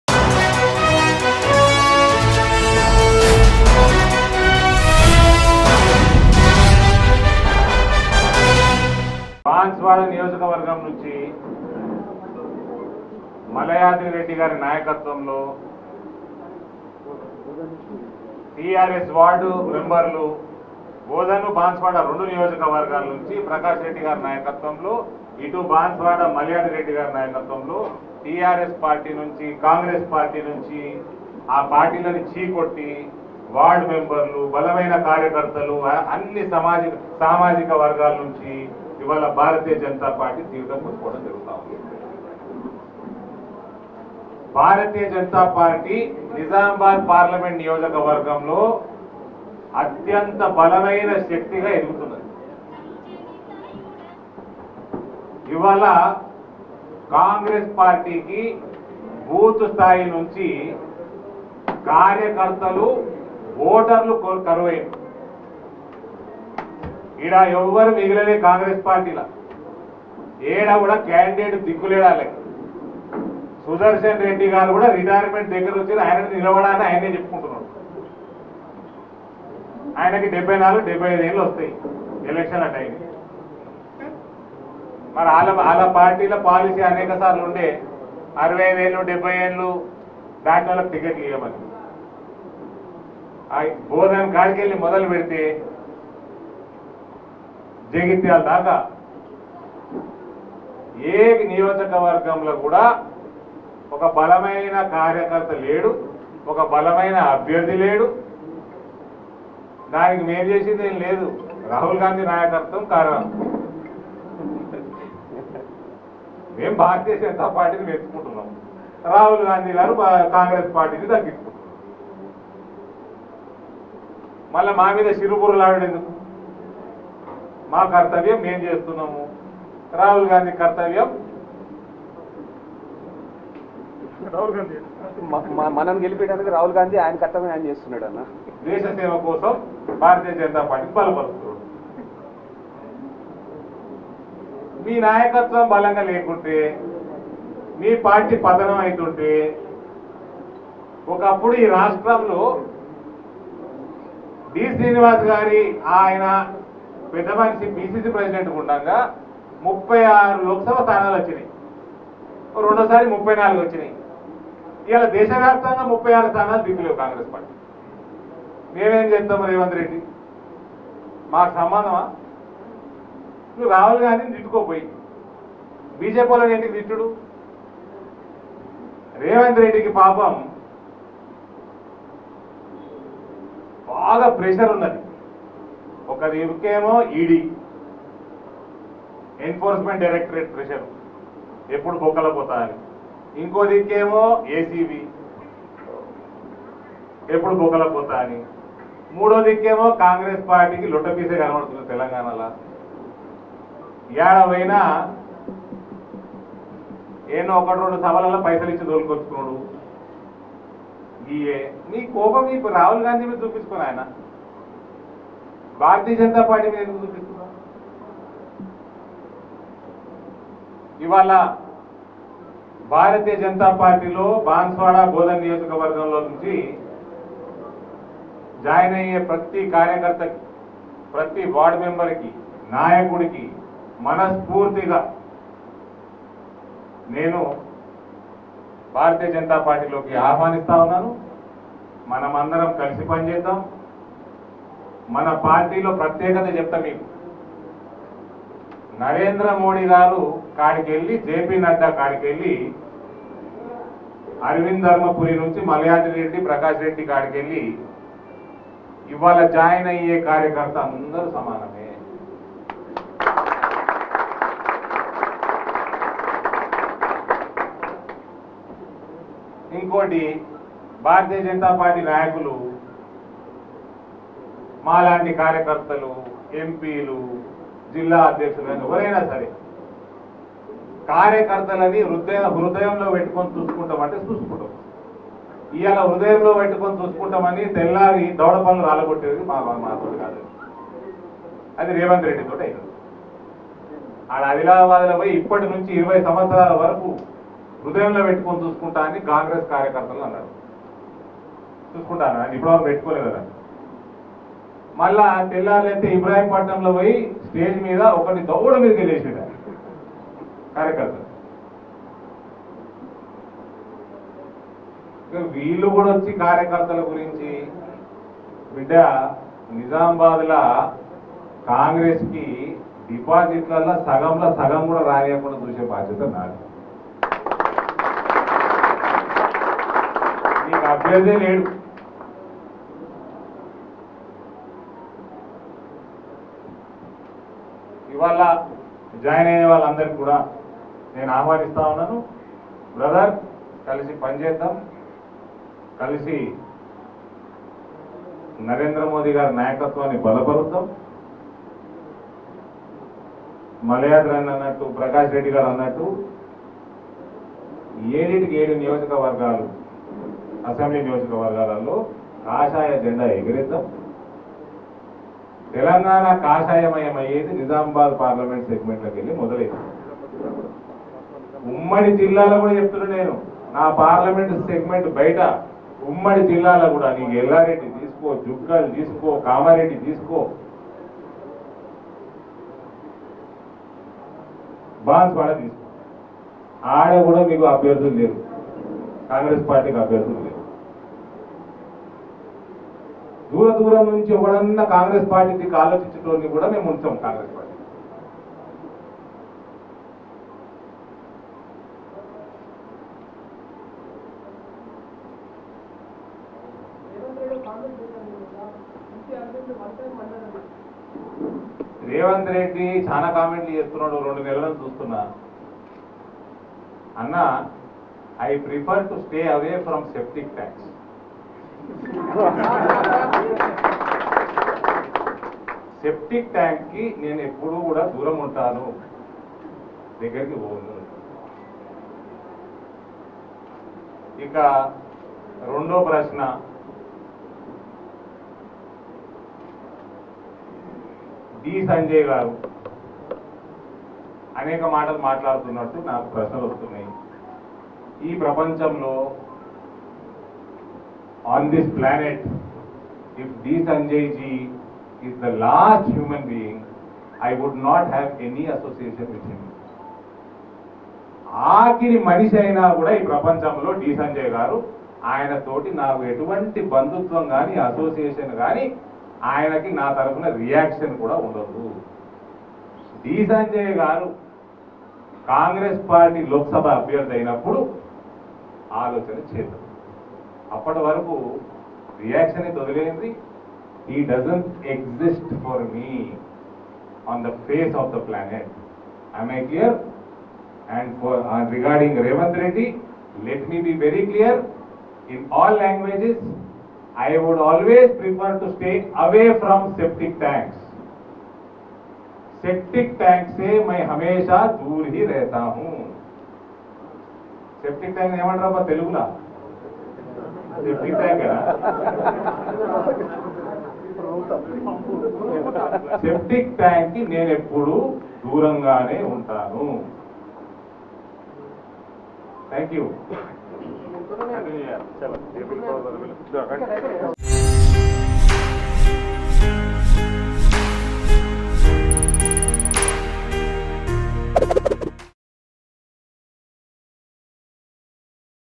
BJP MP Dharmapuri Arvind Satirical Comments On Revanth Reddy | BJP Vs Congress | Mango News
BJP MP Dharmapuri Arvind Satirical Comments On Revanth Reddy.